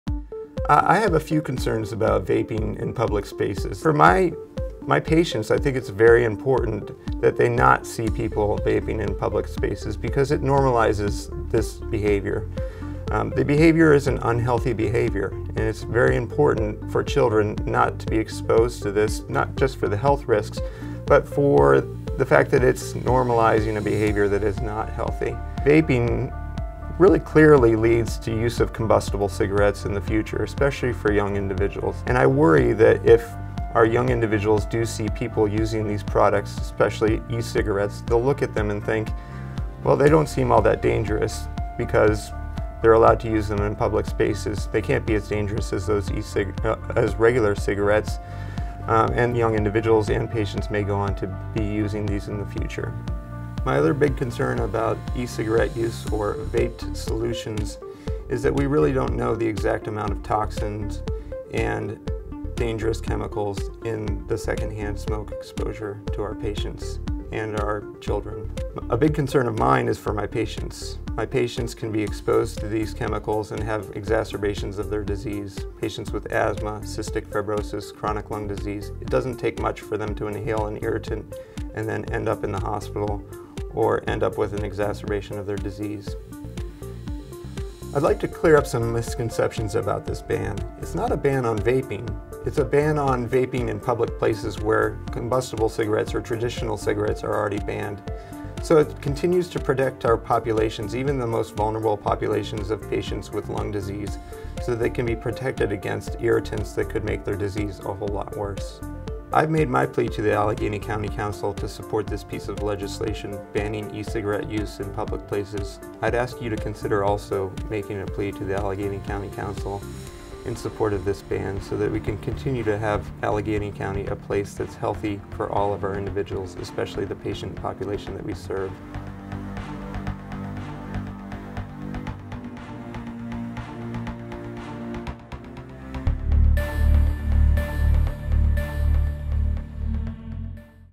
Pediatric Lung Physician Weighs in on Vaping | UPMC
Dr. Jonathan Spahr, clinical director of pediatric pulmonary medicine at UPMC Children's Hospital of Pittsburgh, explains why he supports the proposed Allegheny County e-cigarette regulation.